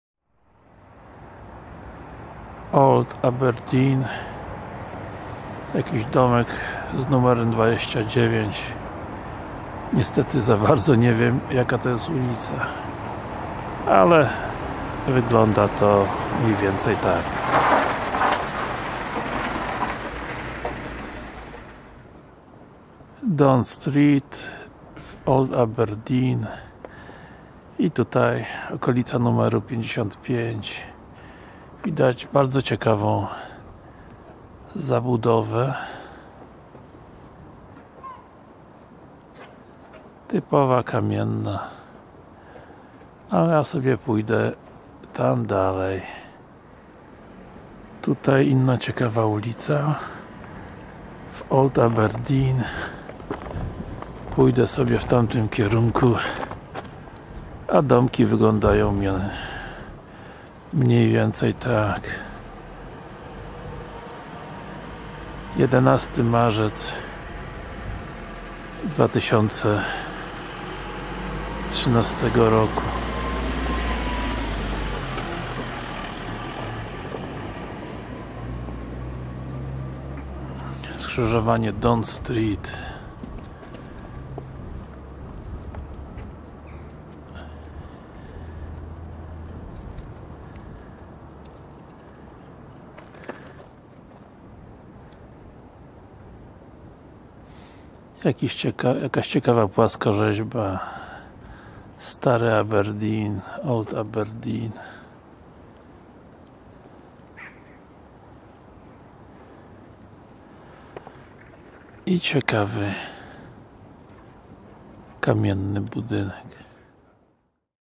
Old Aberdeen Don Street
Wycieczka po Old Aberdeen Don Street i jej okolicach. Zacząłem iść od strony King's College Filmik zrobiony 11 marca 2013 roku. Zabudowa tego miejsca nie zmieniła się od lat. Budynki są zrobione z granitu. Wszystko jest wyjątkowe ciekawe dla turysty z Polski. Dalej poszedłem w kierunku lokalnej Katedry